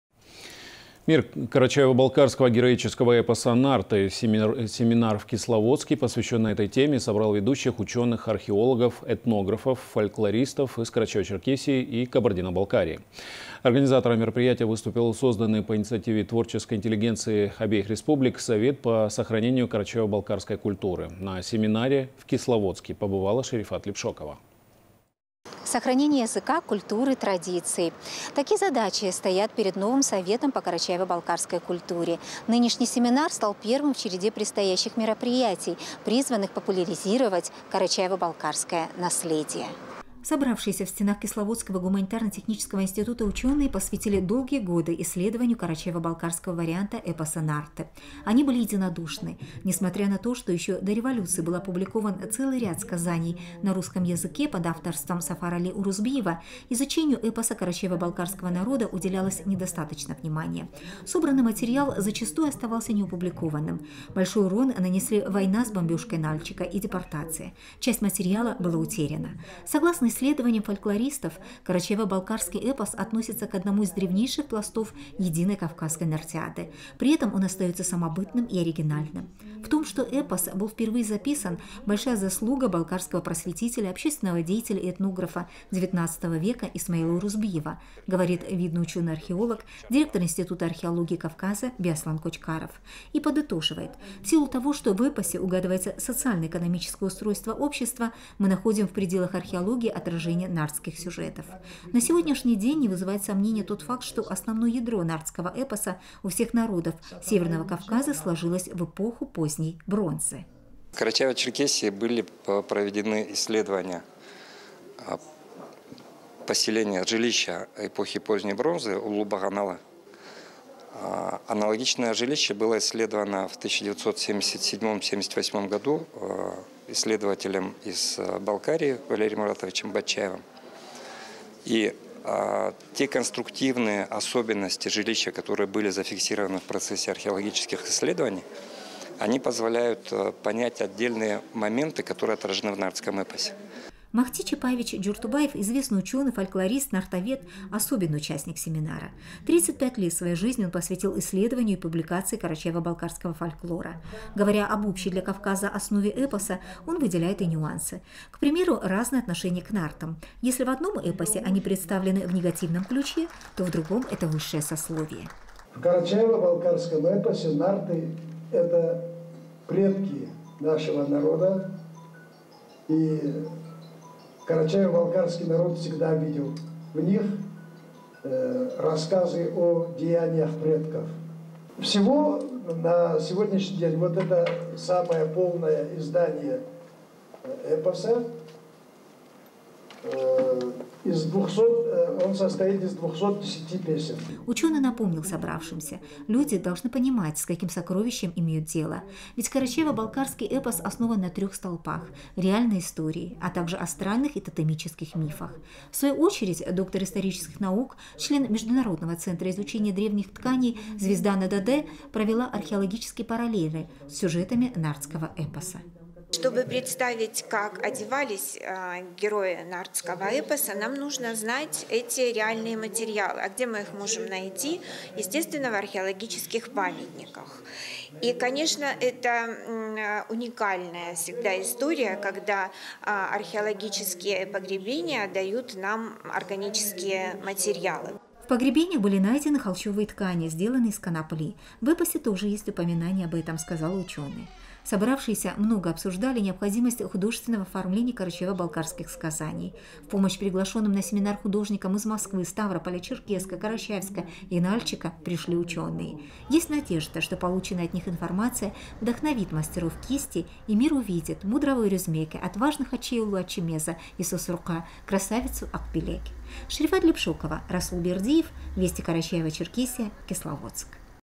В Кисловодске прошел семинар, посвященный карачаево-балкарскому эпосу
Мир карачаево-балкарского героического эпоса "Нарт" - семинар в Кисловодске, посвященный этой теме, собрал ведущих ученых - археологов, этнографов, фольклористов из Карачаево-Черкесии и Кабардино-Балкарии. Организатором мероприятия выступил созданный по инициативе творческой интеллигенции обеих республик Совет по сохранению карачаево-балкарской культуры